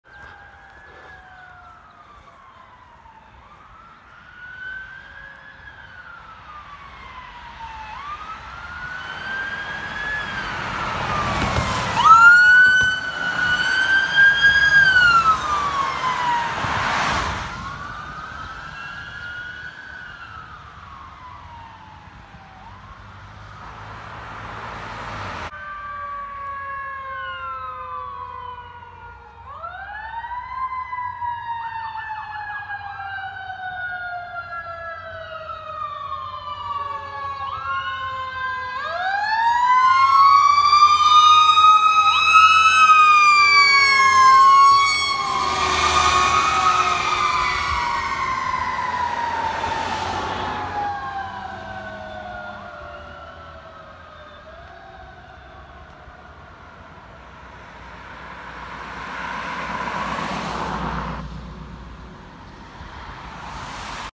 Southington Fire Department Car 4 and Engine 12 + Southington PD responding to an MVA
Filmed 4/16/2021
Car 3 and Squads 1 and 5 were already at another MVA when this one came in, so Car 4 went as Command and Engine 12 responded. You can also see I barely got Southington PD as well. Enjoy!